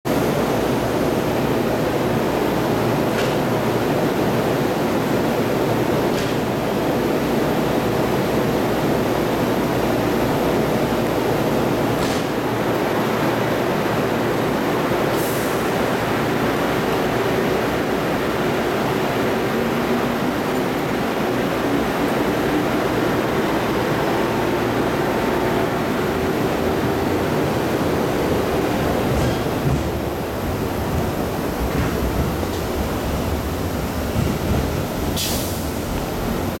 東海道本線３１３系＋２１１系普通列車熱海行き静岡駅発車シーン2021.08.08.
２０２１年８月８日
東海道線静岡地区
３１３系２３００番台W編成＋２１１系５０００番台LL編成
普通列車熱海行き
静岡駅２番線発車シーン
静岡駅２番線にて撮影